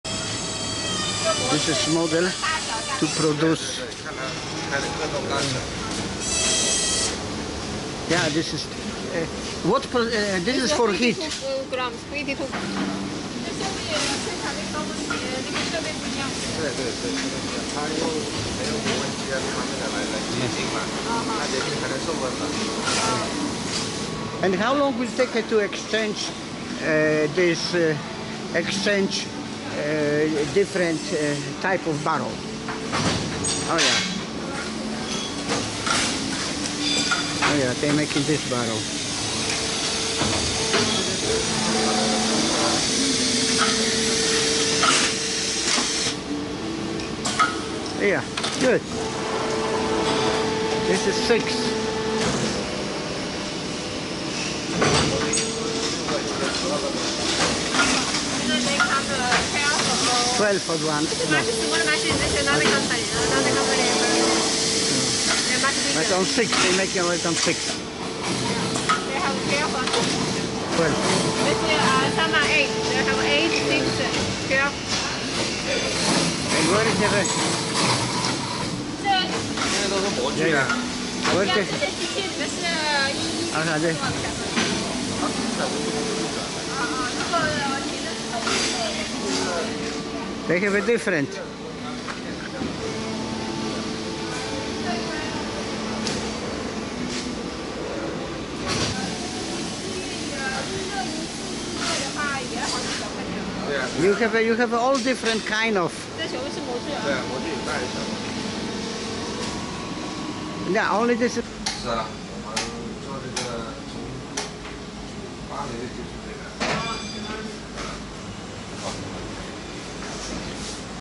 Chiński Fenomen - Fabryczka w Każdym Zaułku
To mi sie podoba w  Chinach - ludzie chcą pracować. Nie oczekują, nie tracą energi i czasu  na unijne zapomogi, ale produkują to, co ktoś inny chce kupić. Zresztą tak samo zaczynałem - brudno, ciasno, ale obrót pieniążkami jest